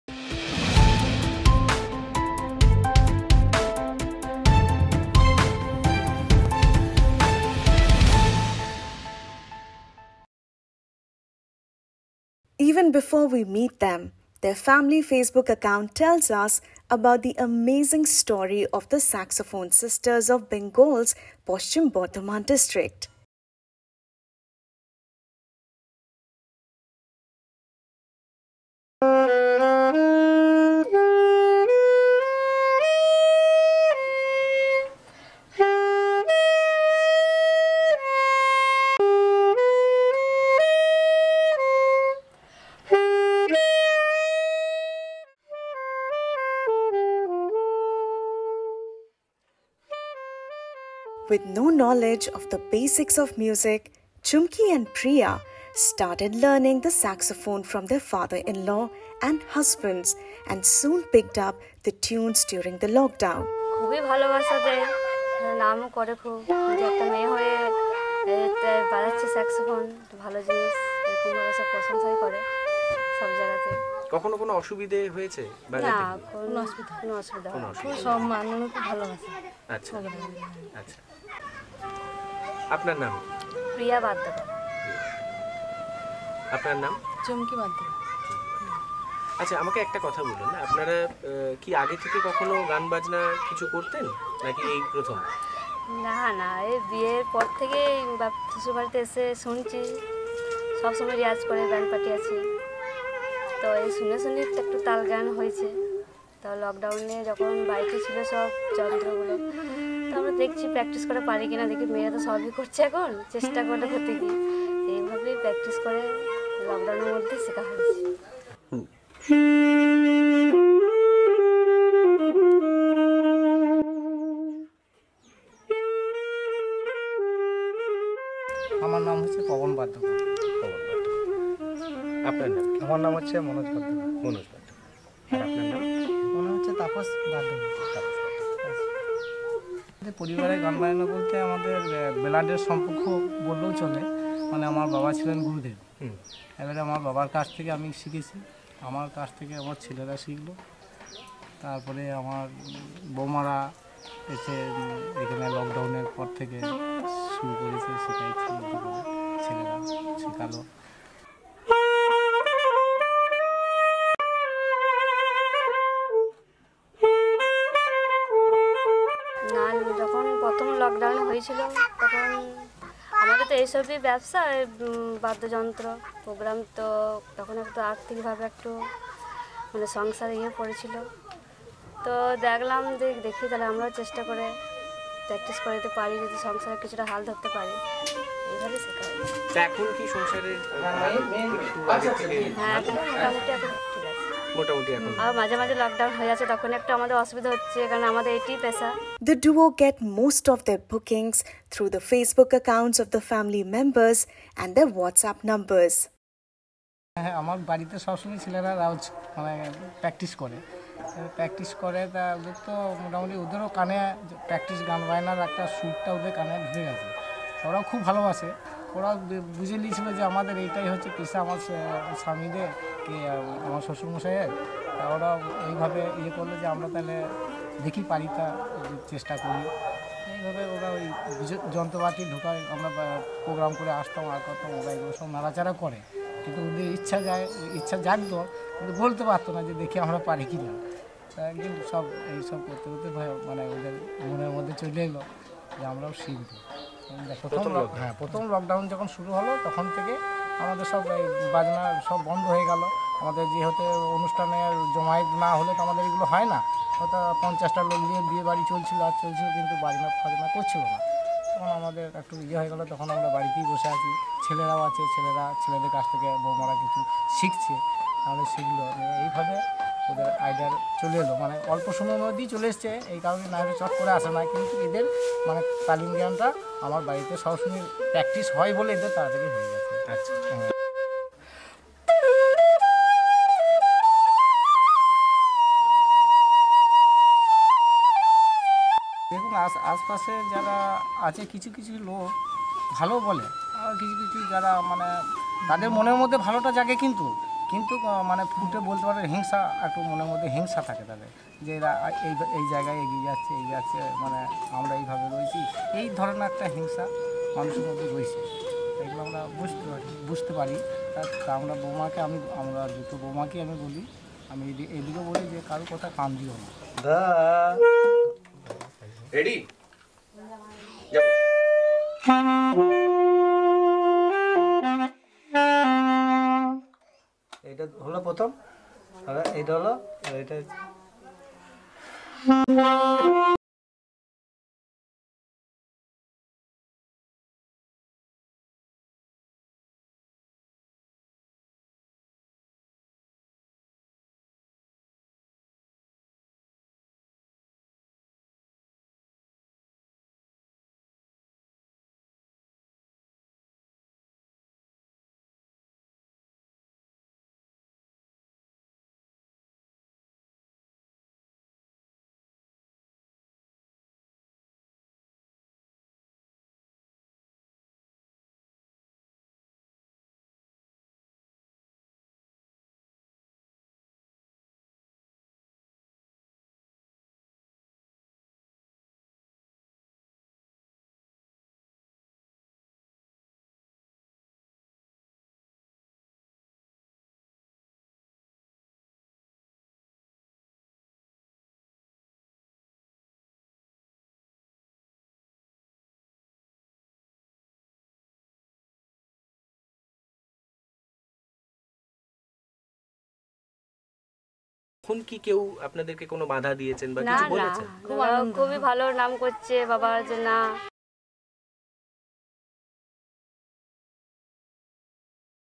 ‘Saxophone Sisters’ — how this Bengal duo overcame digital divide to script a musical journey
With no knowledge of the basics of music, Chumki and Priya started learning the saxophone from their father-in-law and husbands and soon picked up the tunes during the lockdown. They now book events through their Facebook pages and have become a local hit in Bengal's Paschim Bardhaman district.